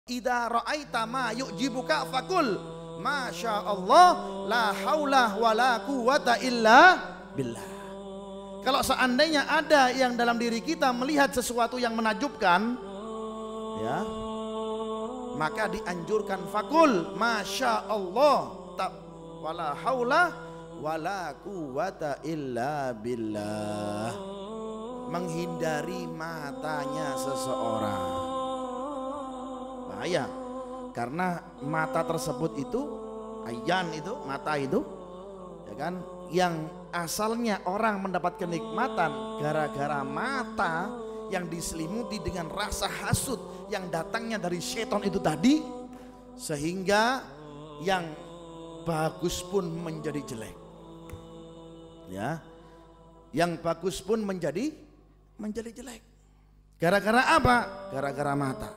Penangkal Dari Mata Orang (Jalsatul Itsnain, 11/3/19) ᴴᴰ | Al Habib Idrus Alaydrus
"Amalan ketika kita melihat sesuatu yang indah nan menakjubkan. Amalan ini pula bertujuan agar tidak timbul yang namanya sifat2 buruk yang datangnya dari setan laknatullah, seperti hasud, iri dsb. Selengkapnya cek video berikut oleh Al Habib Idrus bin Muhammad Al Aydrus"

InsyaAllah Berkah..

majelisrasulullahjawatimur